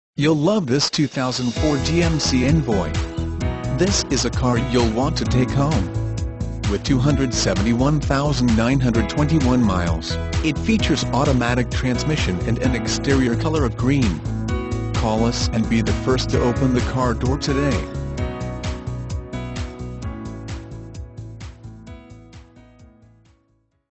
2004 GMC ENVOY Philadelphia, PA N15119XB
2004 GMC ENVOY Philadelphia, PA
Stock# N15119XB

THIS VEHICLE IS BEING SOLD AS IS WITH NO WARRANTY WRITTEN OR IMPLIED.
Recommendations: DRIVERS SIDE VIEW MIRROR ASSEMBLY, RIGHT FRONT STRUT, FRONT DIFF PINION SEAL, TRANSFER CASE RESEAL, 3 TIRES, REAR BRAKES, DRIVE BELT, TRANS PAN RESEAL, CHECK ENGINE LIGHT ON, RIGHT FRONT FOG LIGHT BULB.
Please call for availability.